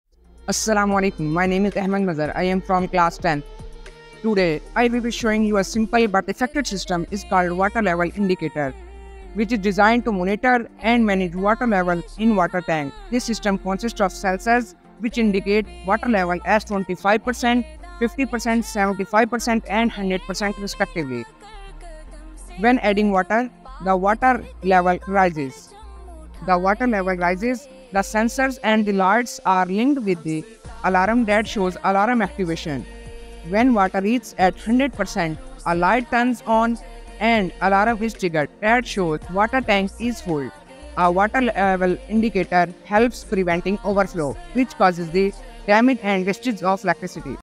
Water Level Indicator Project | DIY Electronics Tutorial (Presented by Ahmad Mazhar) COE Boys M.Garh
In this video, we'll guide you through the step-by-step process of building a simple and effective Water Level Indicator project. Whether you're a student or a hobbyist, this project is perfect for beginners interested in learning about basic electronics and practical applications.

What You'll Learn:
How a water level indicator works.
Required components: Transistors, resistors, LEDs, wires, and more.
Circuit design and wiring explained in detail.
Practical uses for water level indicators (tanks, wells, etc.).